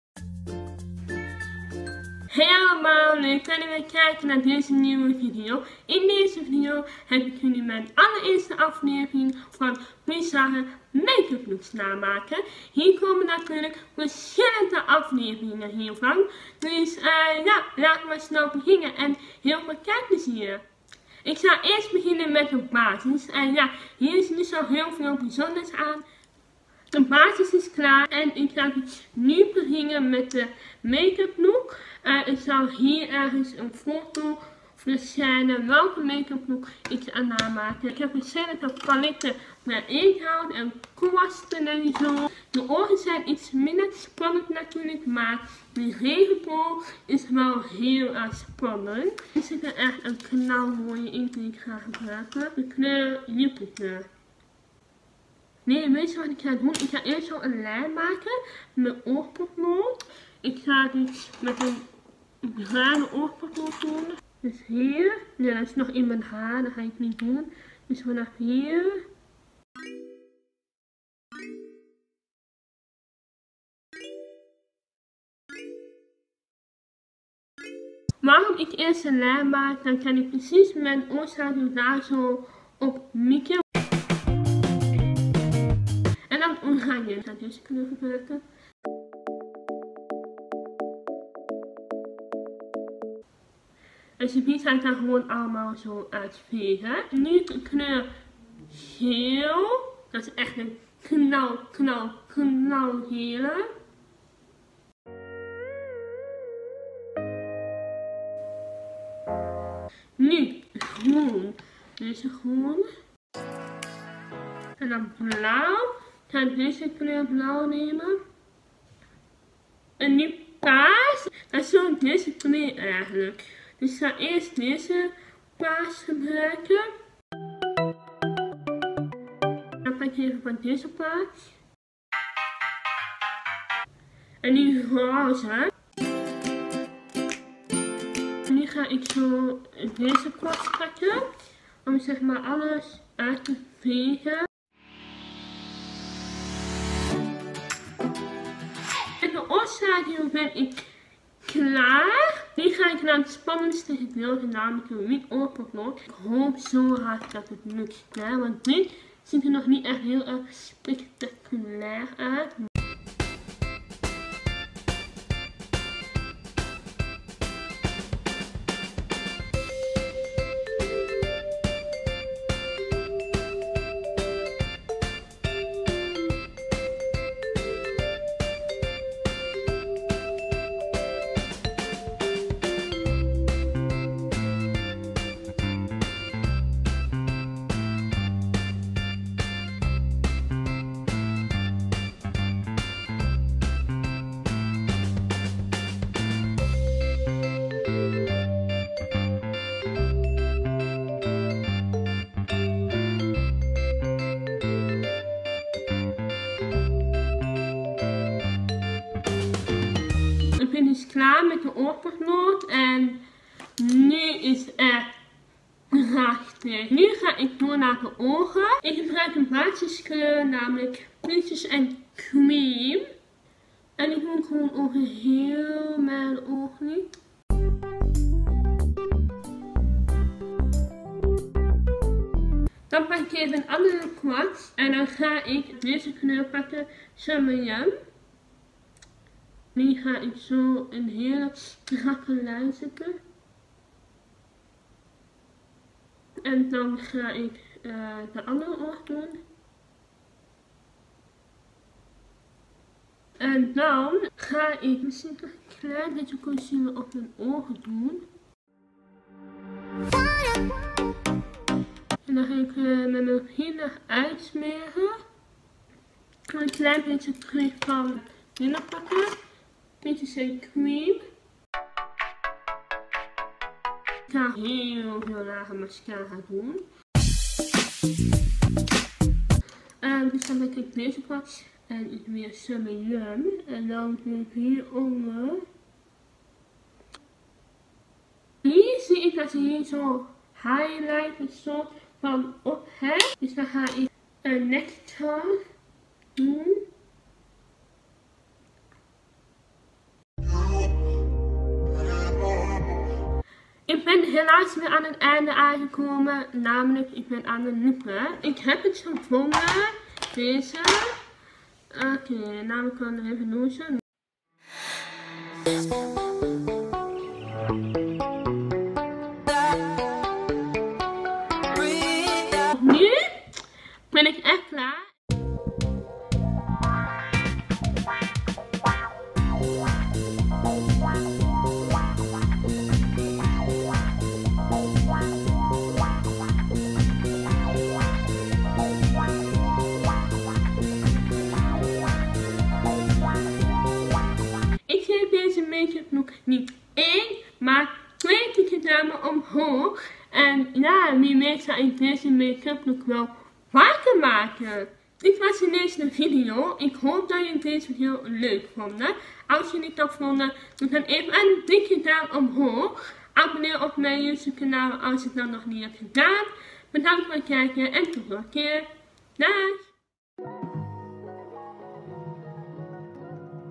REGENBOOG MAKEUP LOOK ⎮ BIZARRE MAKEUP LOOK NAMAKEN #1
Hi! In deze video heb ik mijn allereerste aflevering van Bizarre make-up look namaken #1
Veel kijkplezier!
xoxo botyfully